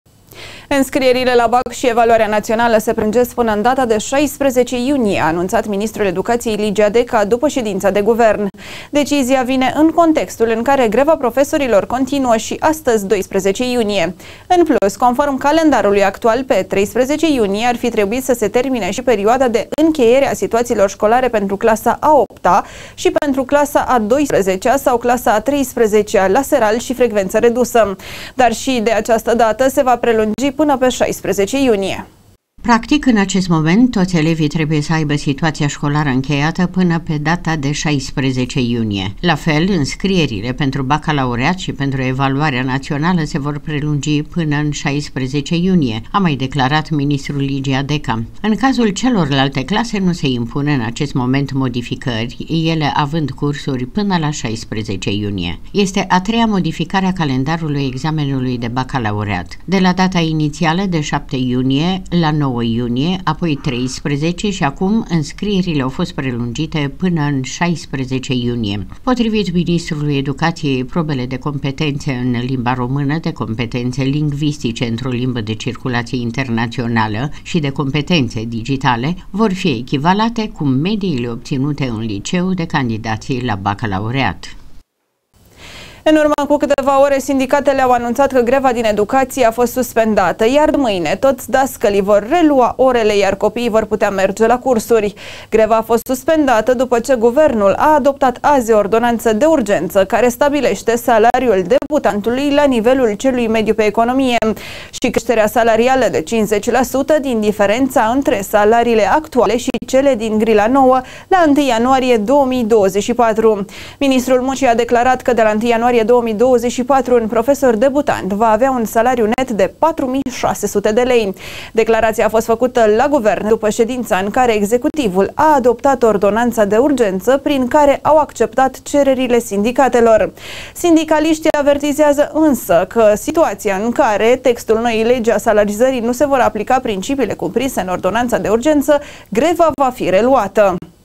O NOUĂ PRELUNGIRE A TERMENELOR DE ÎNSCRIERE
PÂNĂ LA 16 IUNIE, LA BACALAUREAT ȘI EVALUAREA NAȚIONALĂ
       Înscrierile la BAC și la Evaluarea Națională se prelungesc până la data de 16 iunie, a anunțat ministrul Educației, Ligia Deca, după ședința de Guvern. Decizia vine în contextul în care greva profesorilor continuă și astăzi, 12 iunie. În plus, conform calendarului actual, pe 13 iunie ar fi trebuit să se termine și perioada de încheiere a situațiilor școlare pentru clasa a VIII-a și pentru clasa a XII-a sau clasa a XIII-a la seral și frecvență redusă. Dar și această dată se va prelungi până pe 16 iunie.